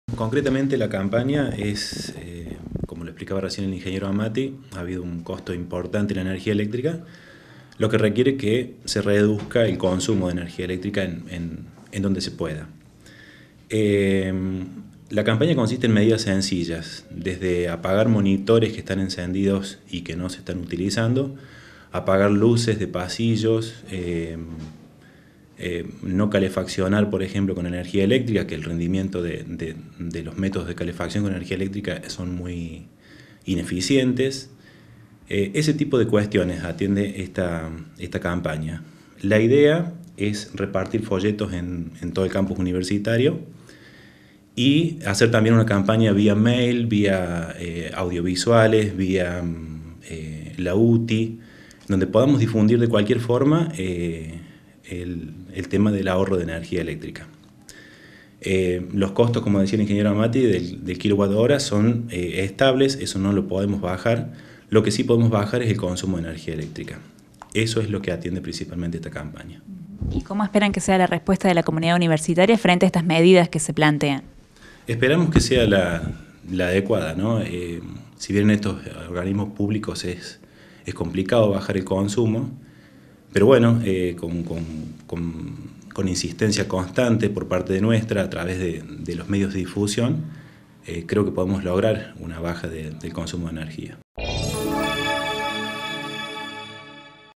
UniRioTV - Ahorro de energía eléctrica - Juan Sanchis
JUAN SANCHIS – INTEGRANTE DEPARTAMENTO DE ELECTROTECNIA. LANZAN UNA CAMPAÑA DE CONCIENTIZACIÓN DE LA COMUNIDAD UNIVERSITARIA.

Facebook.com/uniriotv
Twitter.com/uniriotv
Instagram.com/uniriotv
Web - uniriotv.unrc.edu.ar
o envianos un email a
UniRio TV
Universidad Nacional de Río Cuarto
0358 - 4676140